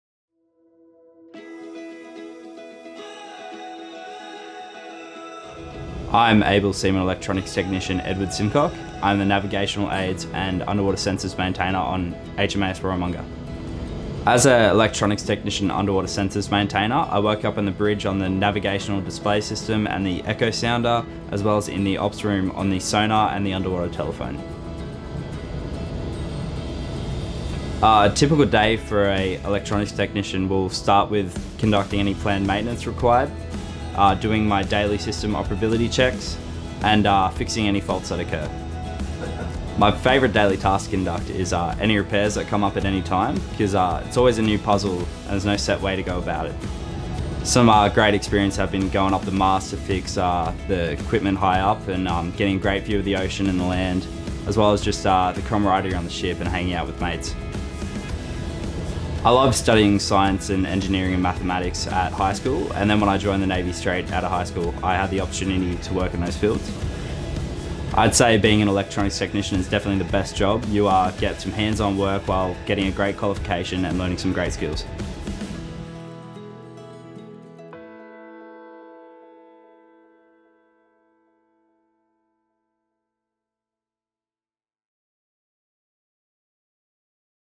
National Science Week - Electronics Technician
National Science Week is Australia’s celebration of science and technology. A technical role in Navy can provide a fulfilling career for those with an interest in Science, Technology, Engineering, and Mathematics (STEM).

Able Seaman Electronics Technician Edward Simcock is the navigational aids and underwater sensors maintainer aboard frigate HMAS Warramunga.

Able Seaman Simcock enjoyed studying STEM in school and has found a career in Navy that appeals to his love for finding out how things work and the satisfaction of finding and fixing faults.

V20222441